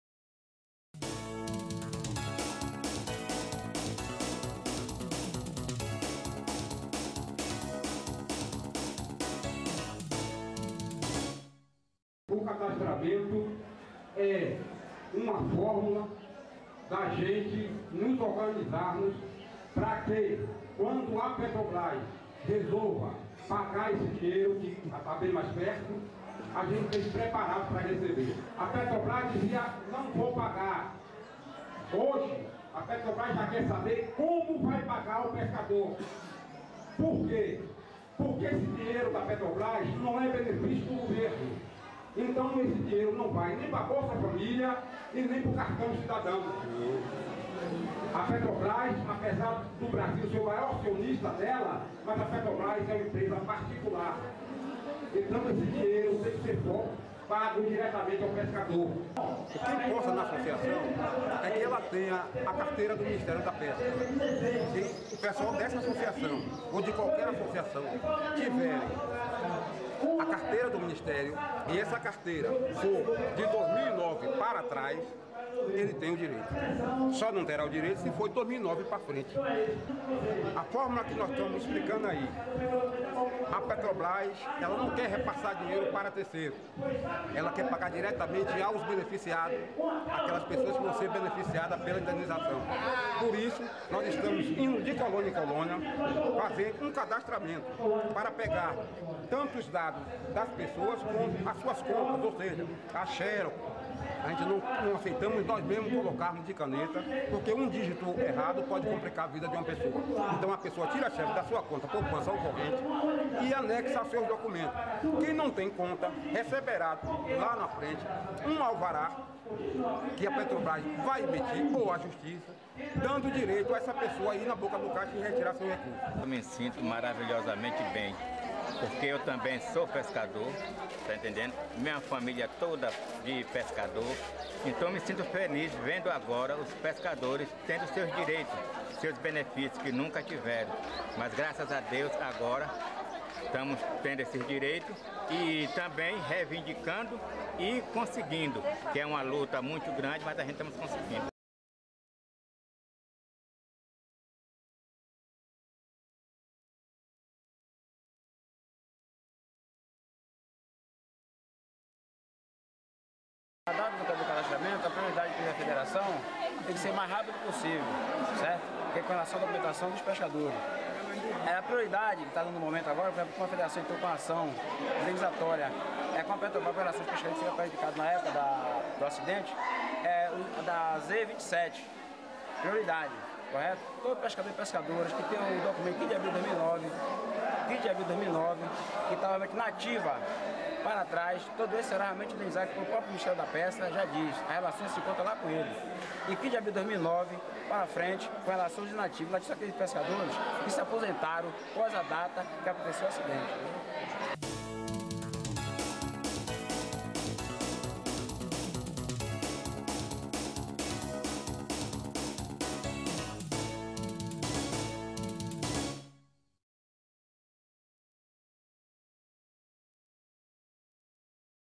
Z-27 Parte 1
Pagamento de indenização a pescadores da Federação de Pescadores e Marisqueiras/os de Acupe.
 Hoje dia 12 de novembro de 2011, na colonia de pescadores Z-27 da comunidade de Acupe, aconteceu uma reunião, que se fez presente toda comunidade pesqueira de Acupe, onde foi esclarecido todo o procedimento para o recebimento da indenização da Petrobras decorrente ao pagamento de indenização a pescadores da Federação de Pescadores e Aquicultores, por causa do vazamento de óleo na Refinaria Landulpho Alves, em São Francisco do Conde, no Recôncavo Baiano. O recurso é retroativo a abril de 2009, mês do acidente.
 Apesar da expectativa dos pescadores e marisqueiras, o presidente da Federação não se fez presente.